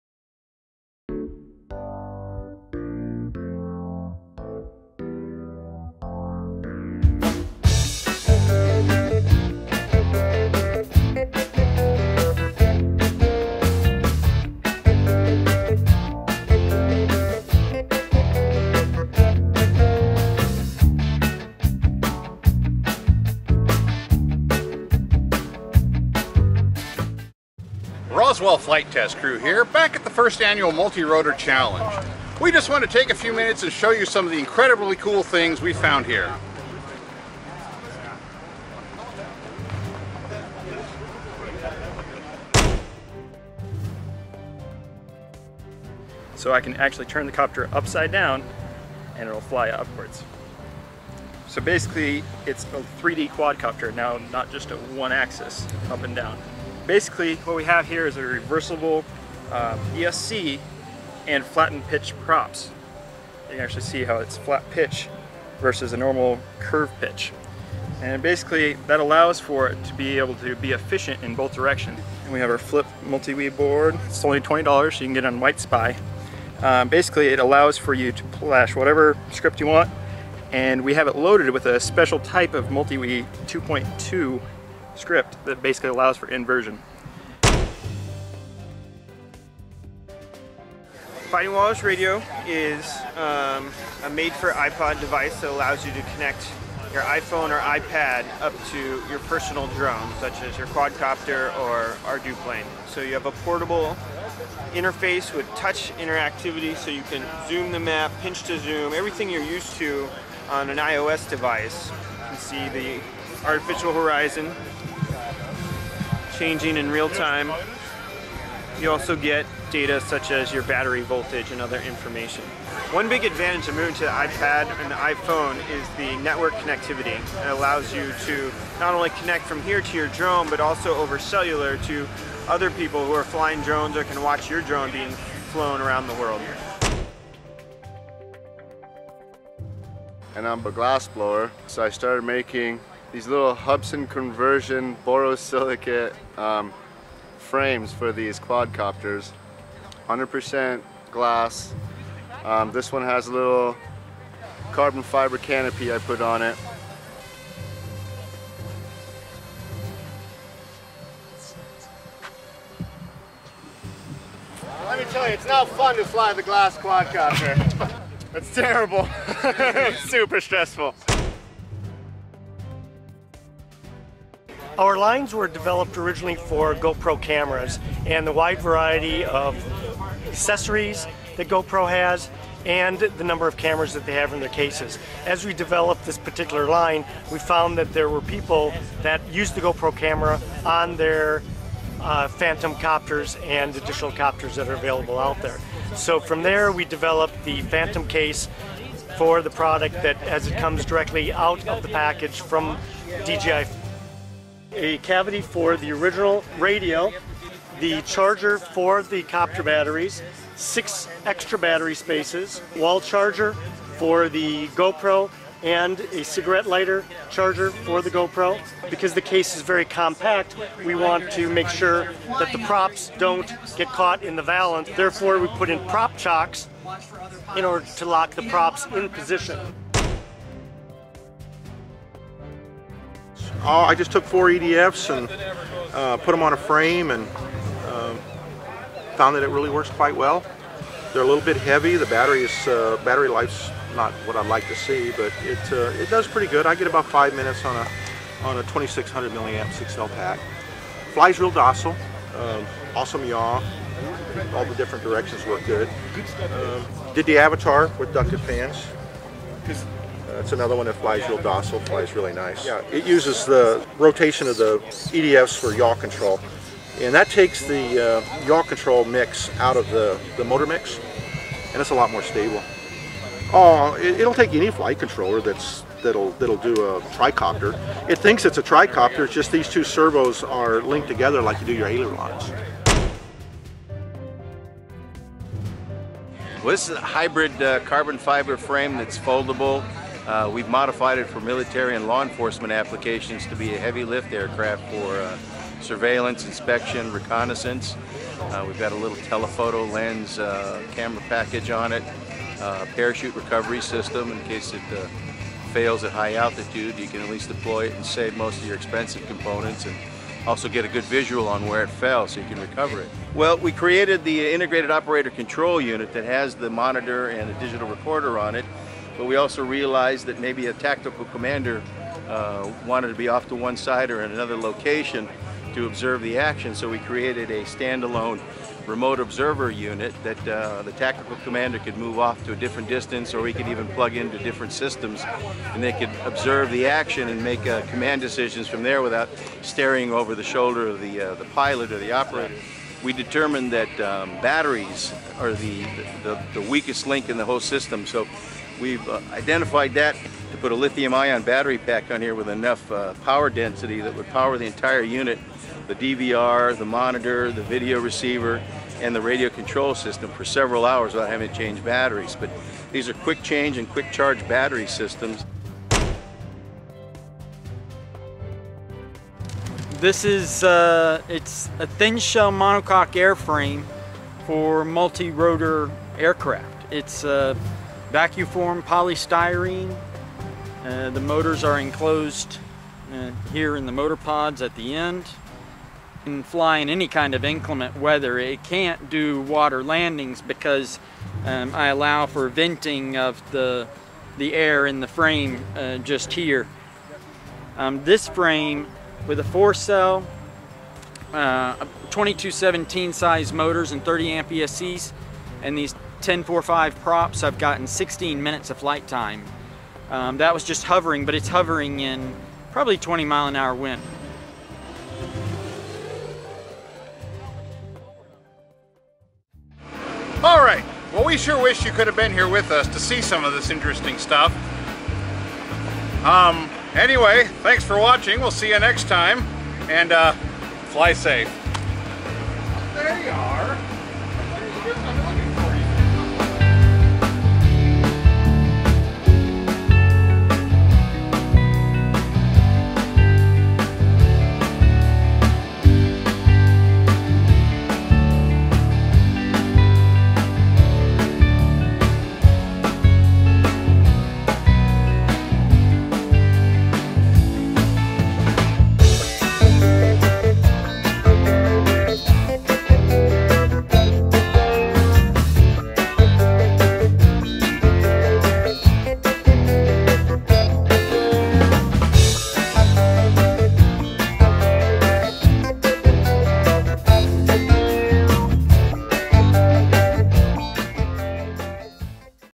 RFTC: Innovations and other Cool Stuff at the 2013 Multirotor Challenge
In this episode, the Roswell Flight Test Crew revisits the First Annual Multirotor Challenge in Fallbrook, California -- hosted by Innov8tive Designs. With amazing technology and cool, new stuff on display throughout the event, Techinstein and Lucidity talk to a few of their fellow pilots and the vendors about their contributions to this fast-changing field.